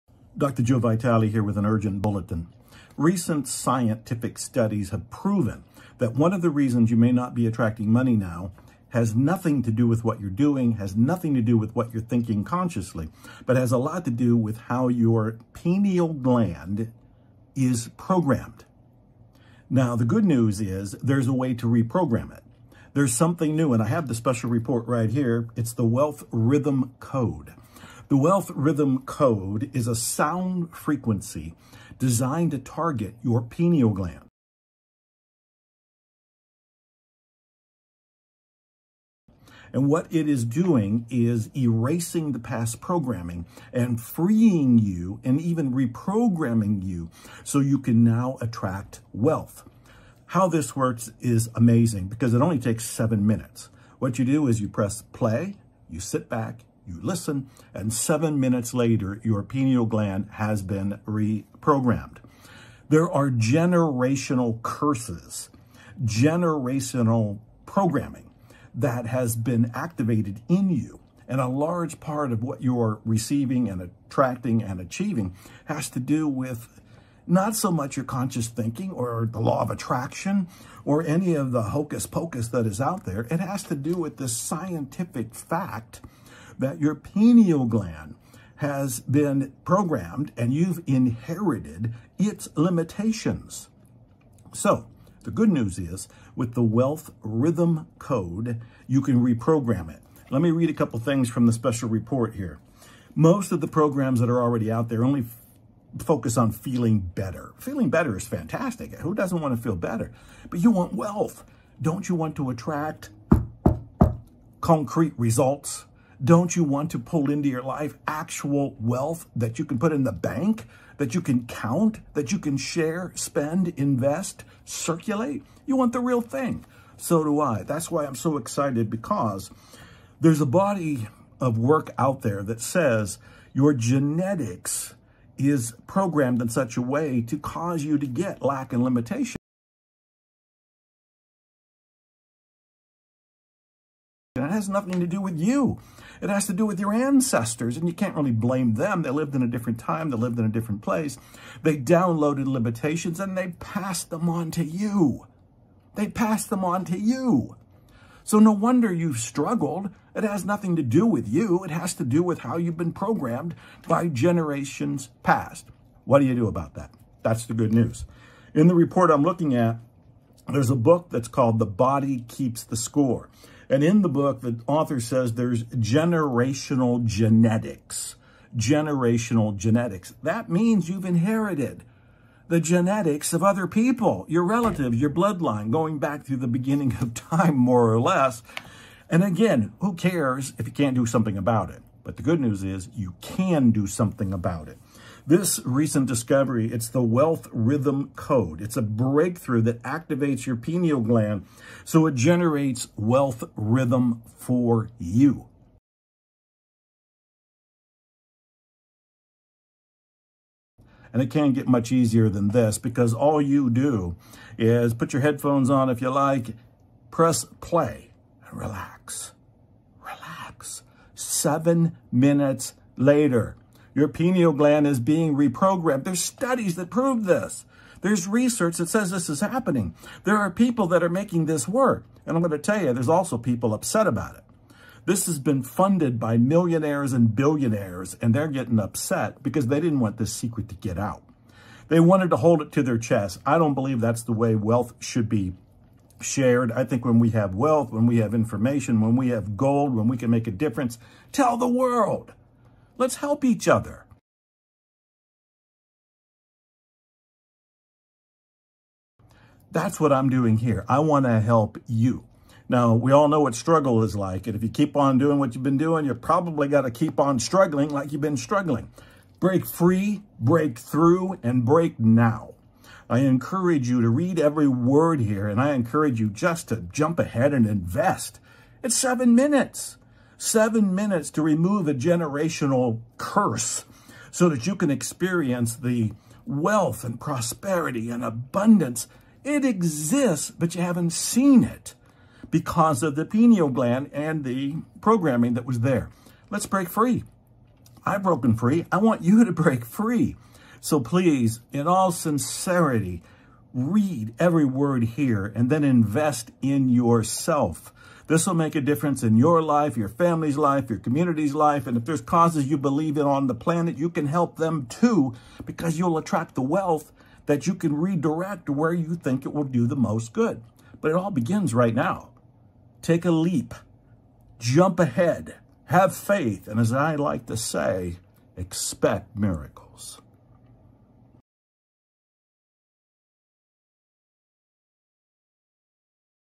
Wealth Rhythm Code by Dr. Joe Vitale - Wealth Rhythm Code Review - Dr Joe Vitale Wealth Rhythm Code
The Wealth Rhythm Code is a unique sound frequency developed using proven science to uniquely target your pineal gland and generate Wealth Rhythm, which is key to attracting more wealth.